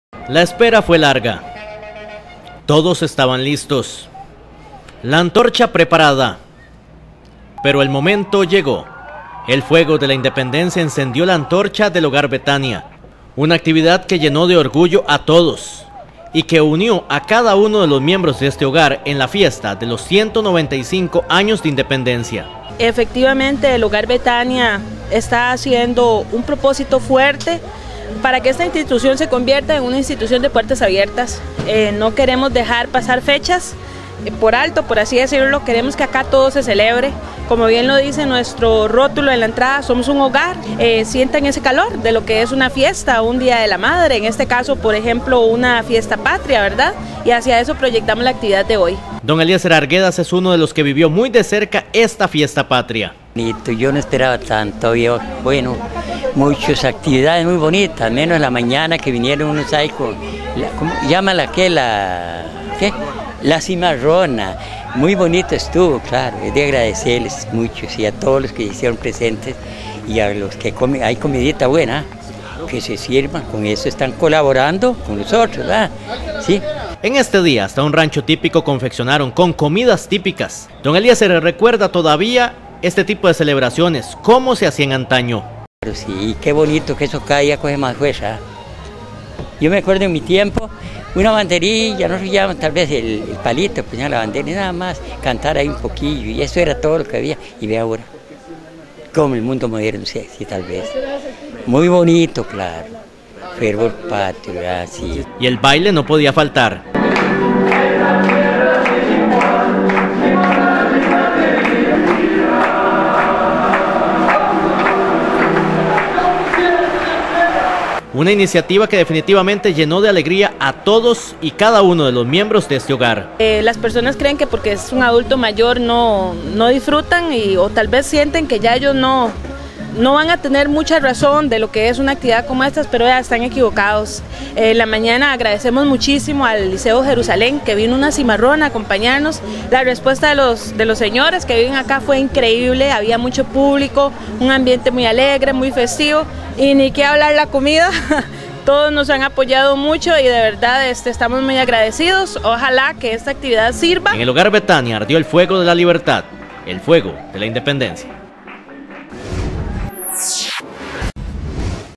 Hogar Betania celebró la independencia con la llegada de la antorcha
En el Hogar Betania vivieron el fervor patrio con la llegada de la antorcha, esto fue parte de una celebración patria que organizaron para  todos los miembros de este hogar.